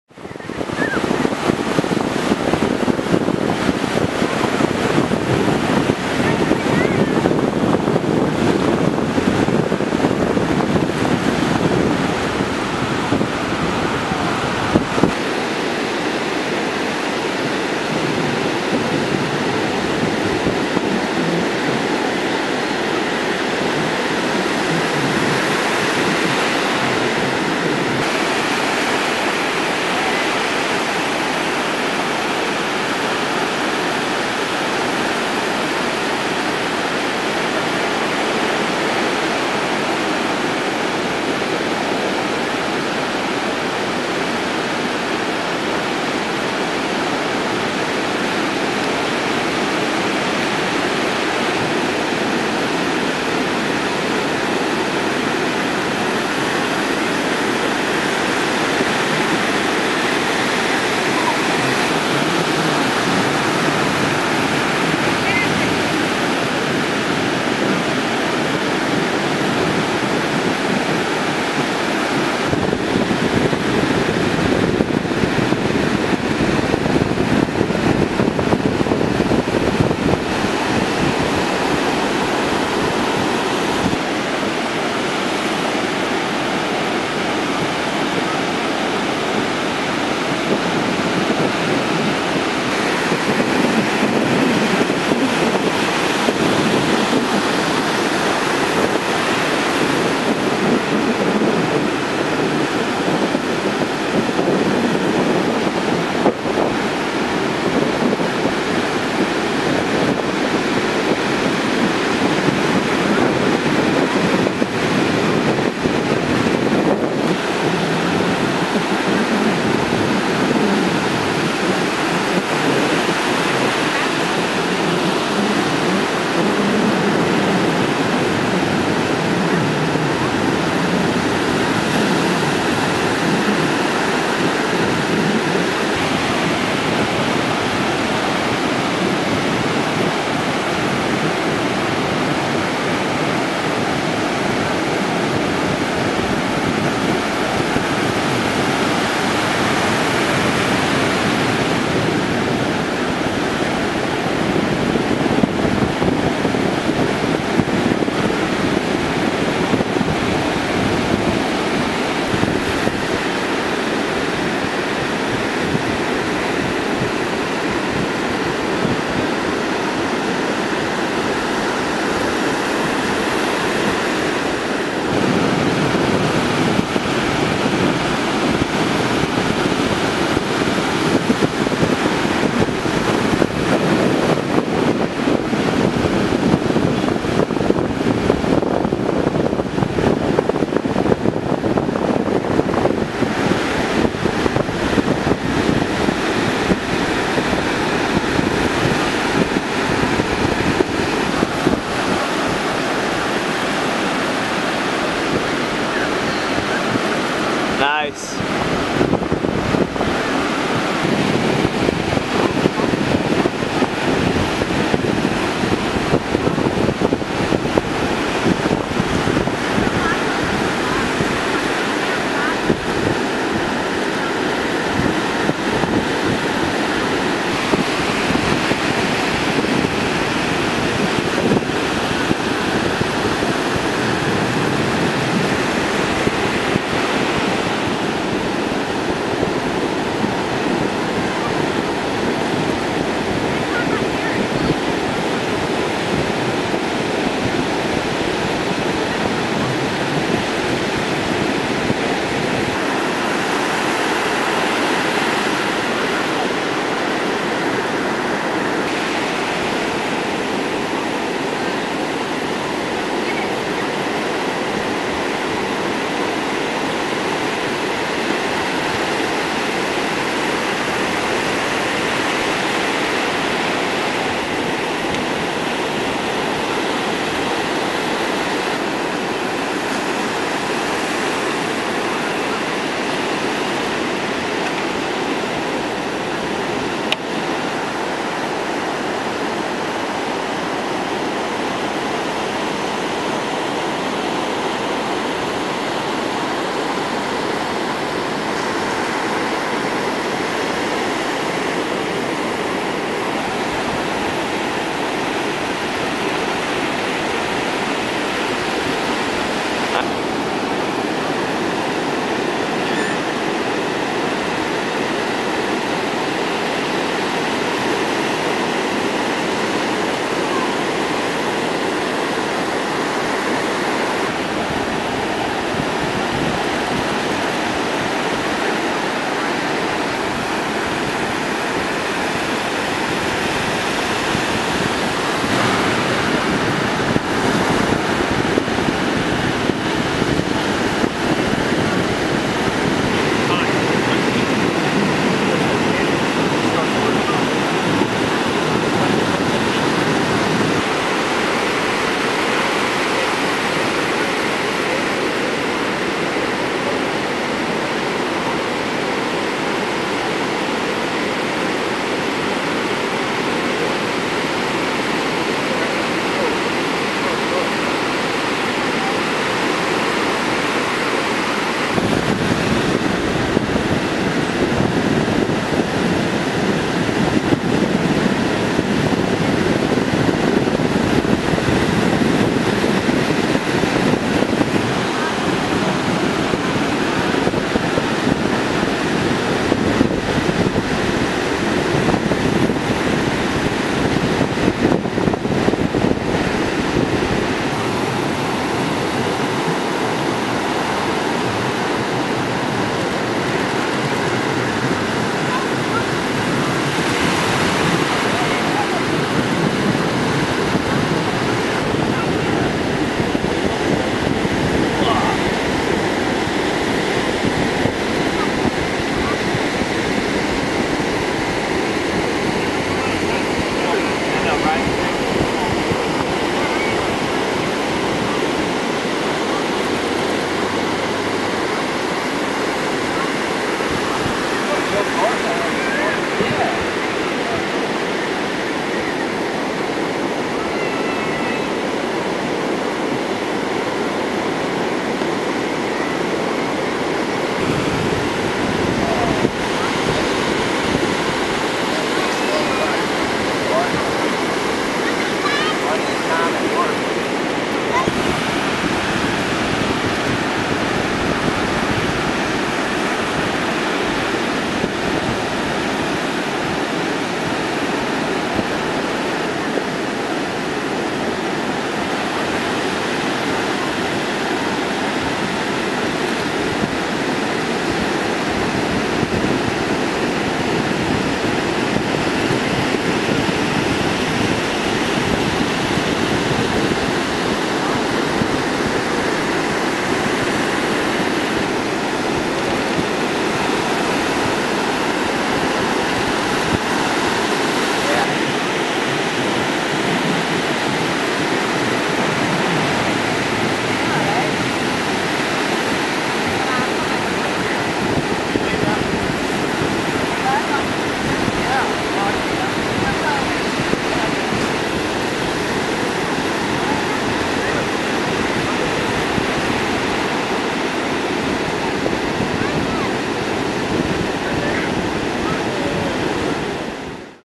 Hurricane Isaac Surf in South Padre Island, Texas
Hurricane Isaac was kind enough to bless us with some surf in SPI, Texas!